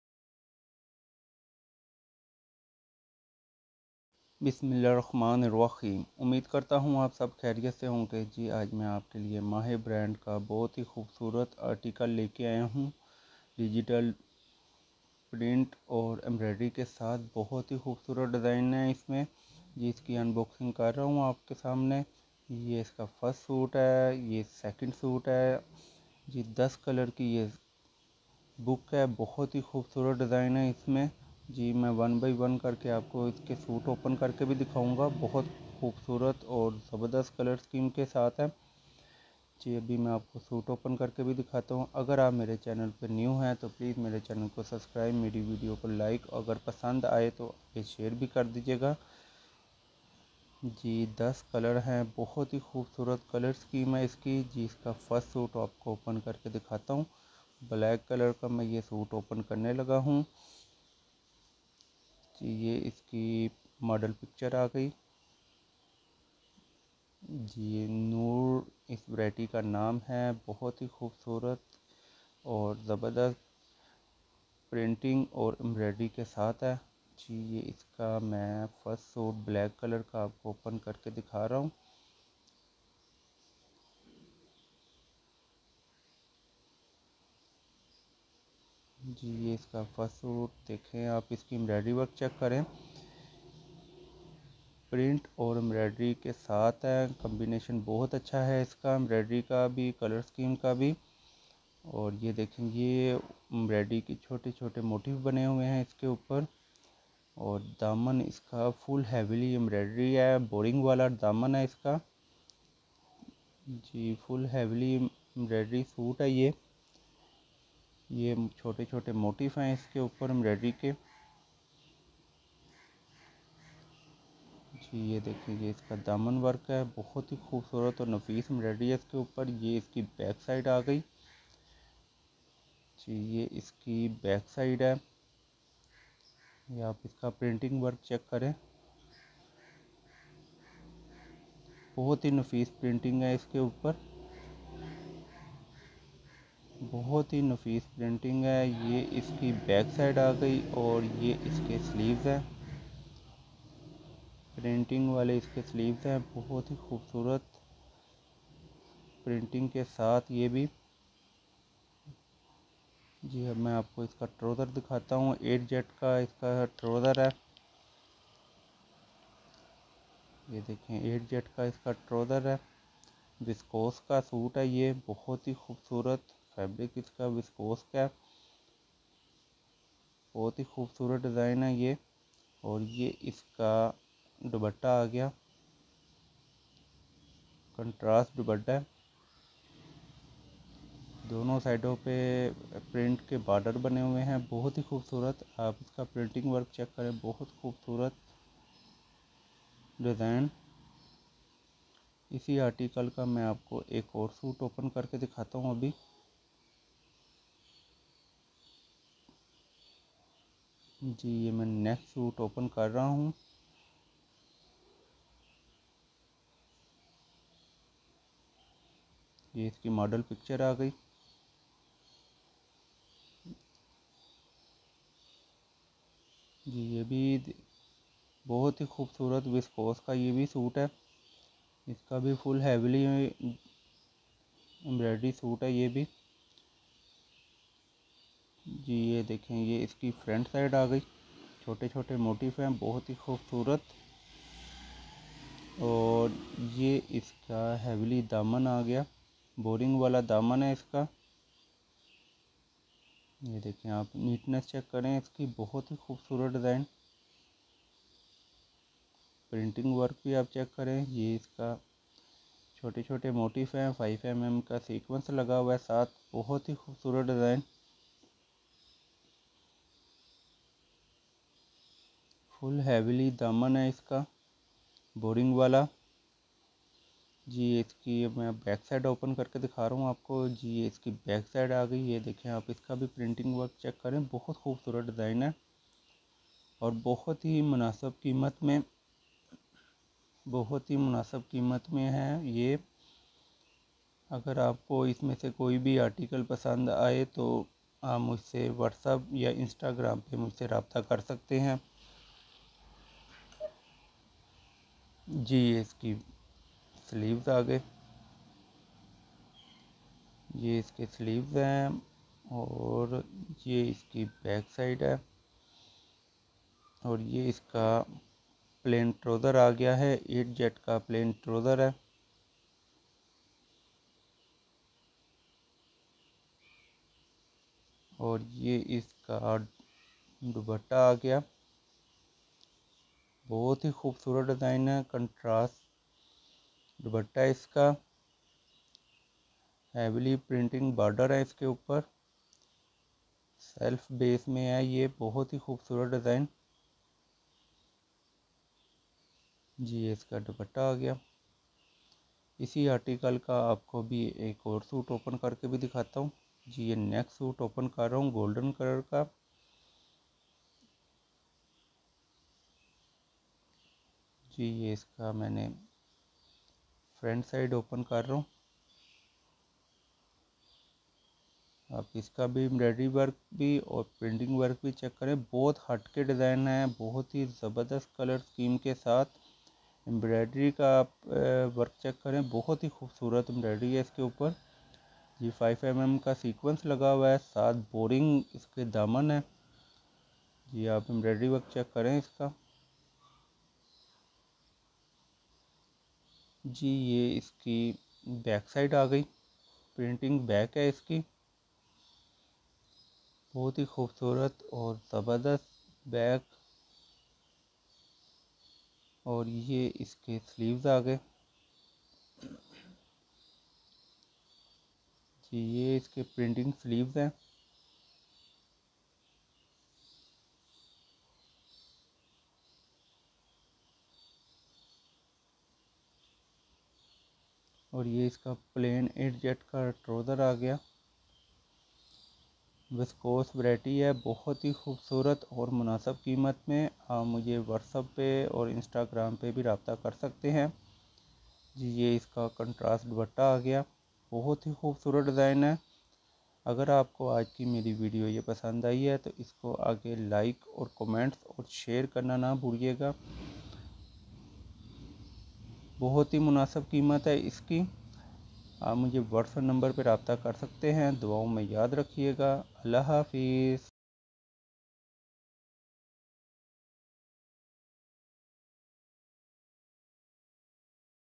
Viscose Embroidered Suits Part 1 | Winter Collection | Mahay By Saqi Arts | Abdul Hadi Hassan
Viscous new collection aa gai hai.Viscous ki ya book 10 designs or 10 rang ki hai. Bhot he khoobsurat printing or embroiderer ky sath.  ager ap ko is ma se koi be suite pasand ay tu ap muj ko whats up ya instagram per rabta ker sakti hai. Fabric ki grantry hai. InshaAllah ap ko ya design pasand ay gy. Bhot he soft or acha fabric hai .

support me..Thanks

viscous ky new design
viscous suit
viscous 2024
embroiderer viscous design
online shopping